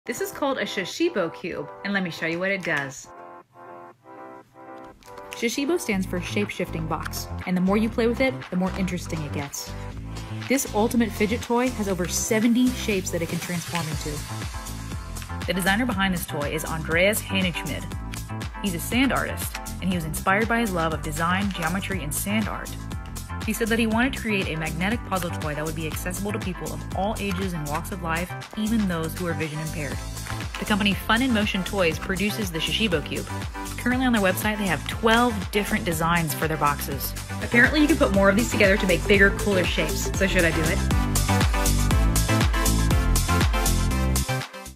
What is Shashibo - Shape Shifting Puzzle Box Toy?!
Check out all of our amazing Shashibo designs to choose from available on our

Unlock the mystery of Shashibo - the shape-shifting box! Just 1 magically mysterious Shashibo transforms into over 70 shapes. With 36 rare earth magnets in each cube, you can combine several Shashibos to expand your puzzle cube into another dimension. Build larger geometric shapes and create unlimited possibilities as you add cubes. Collect and connect them all! Can you master Shashibo?